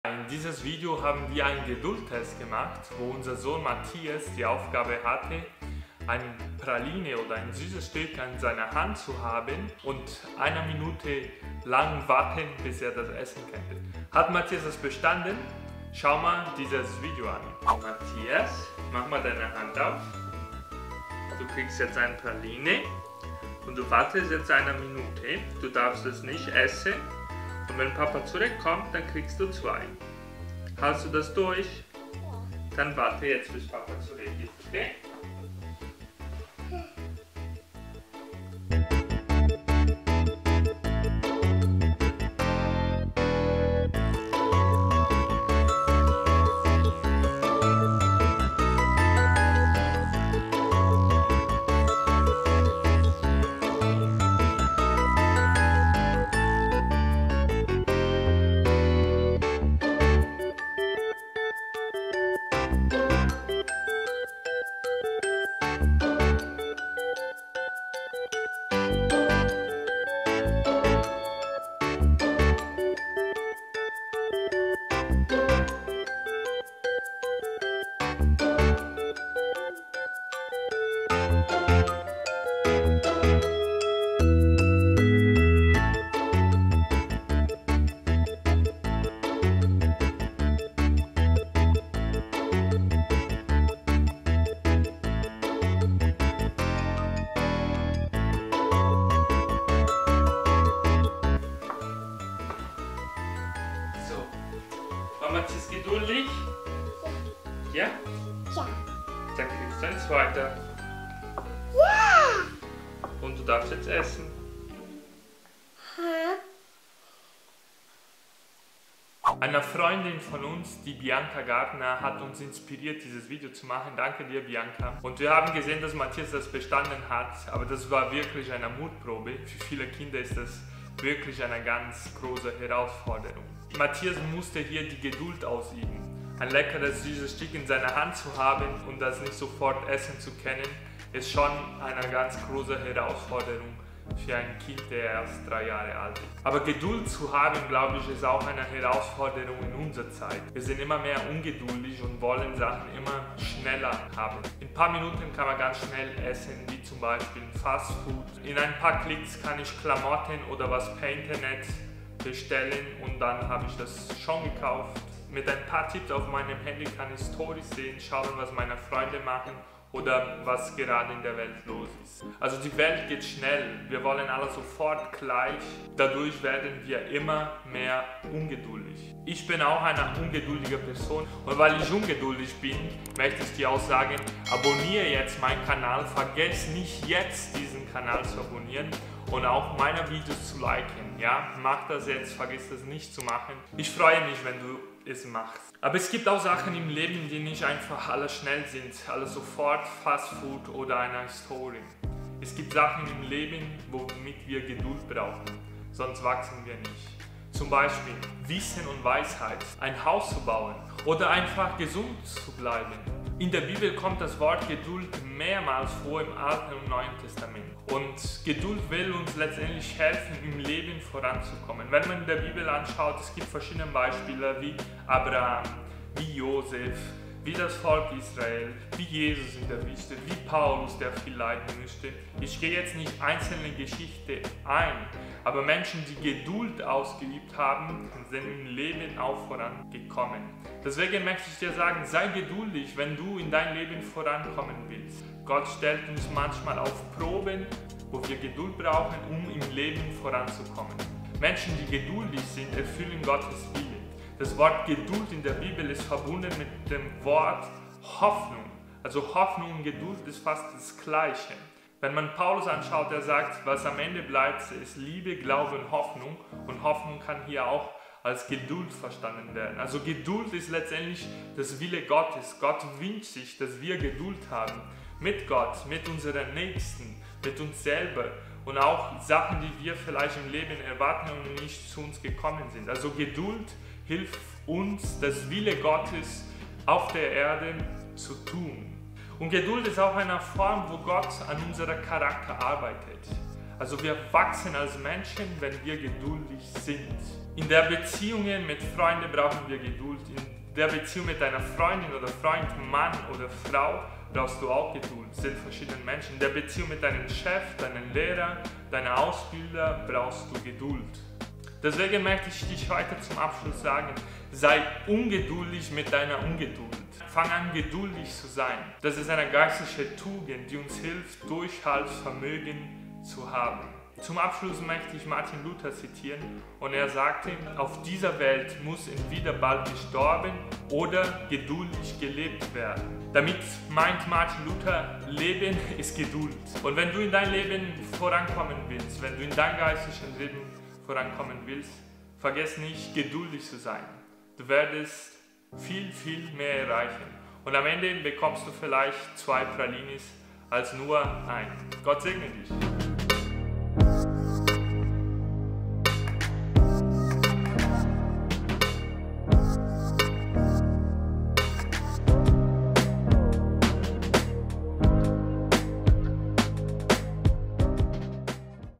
Wie geduldig bist du?
Mein Sohn Matthias macht in dieses Video einen Geduldstest. Außerdem erkläre ich wie es wichtig ist, geduldig zu sein. Wie geduldig bist du?